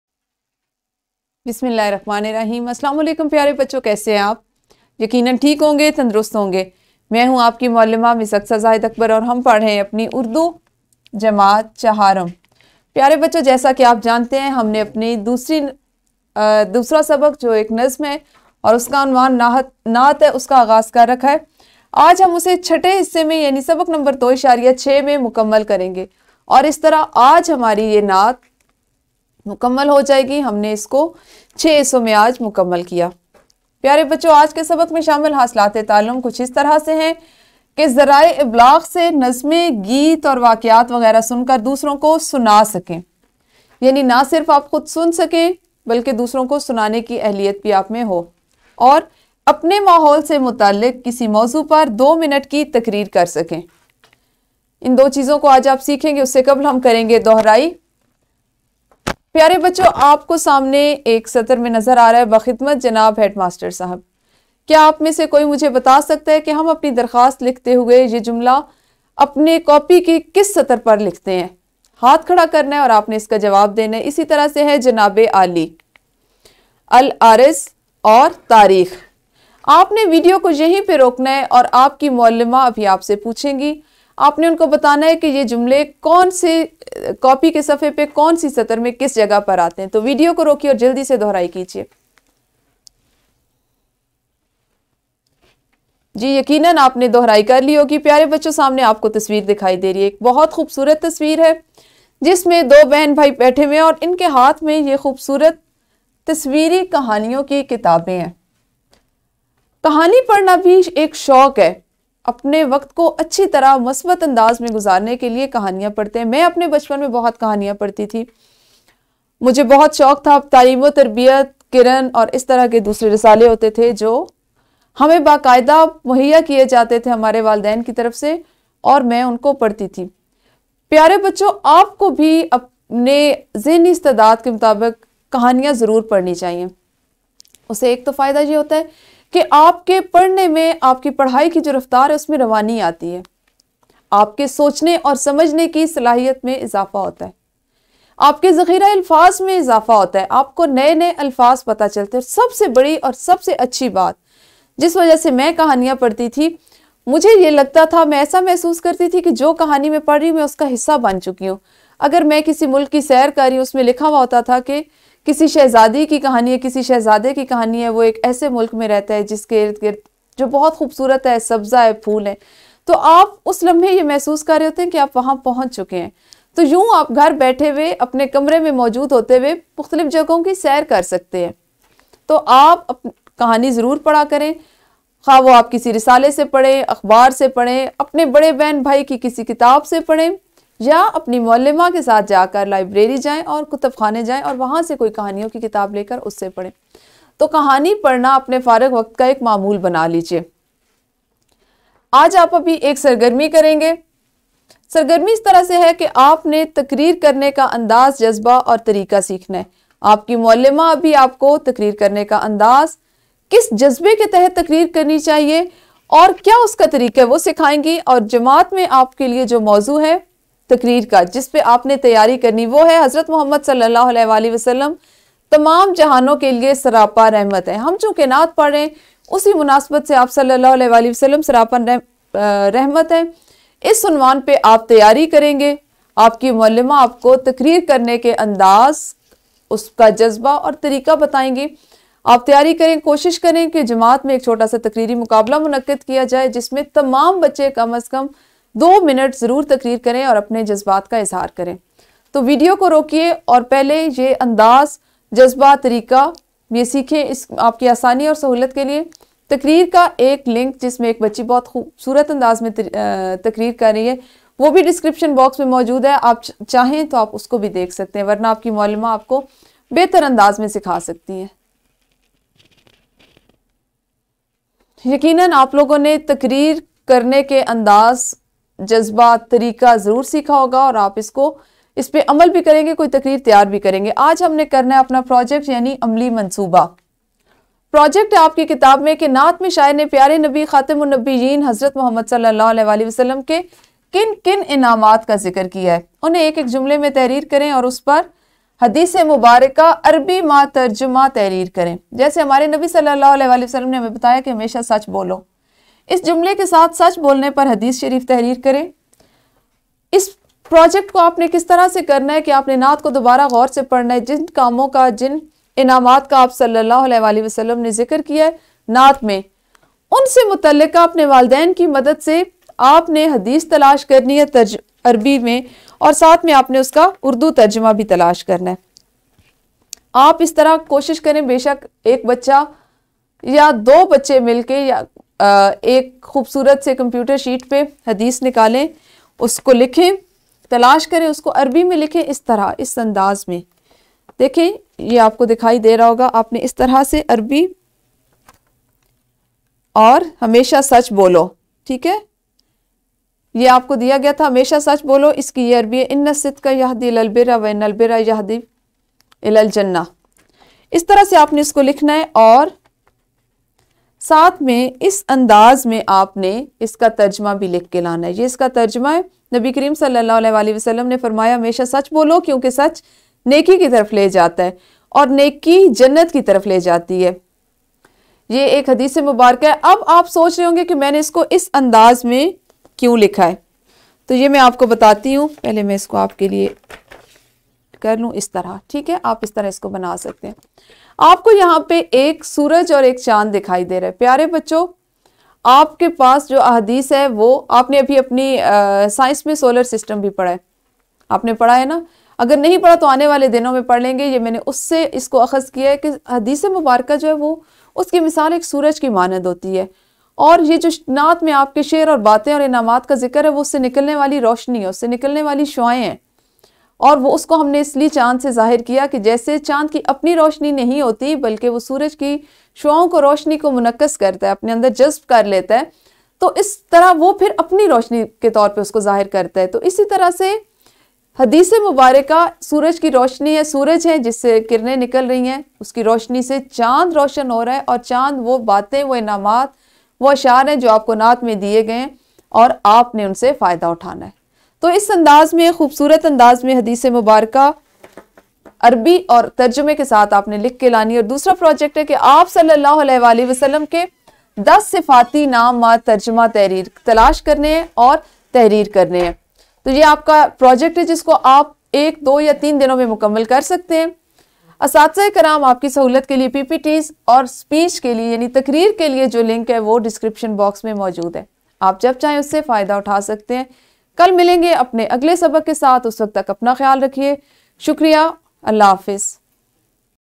Urdu Grade 4 Unit 2 Lecture 6 || SNC 2020
Es sabq ki tadrees ky bahd talbah es qabil ho jain gy keh wo:
# Apnay mahol sy mutaliq kisi mauzoo pr do mint ki taqreer kr sakain.

1. PowerPoint Presentation

2. Lesson Plan